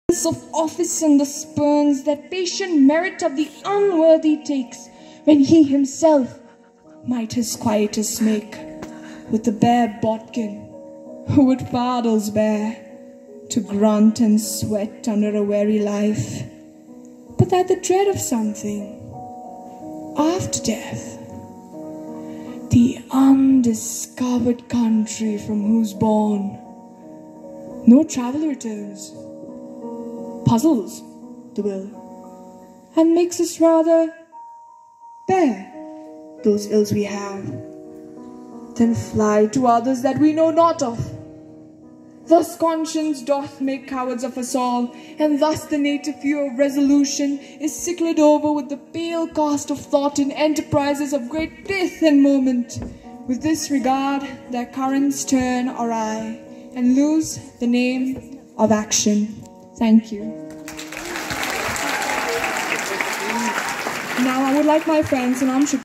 Shakespeare Day ,23rd April
"To be or not to be " from Hamlet recited by Tshering Norkey of class 10 ' B'.
Tashi Namgyal Academy Celebrates Shakespeare Day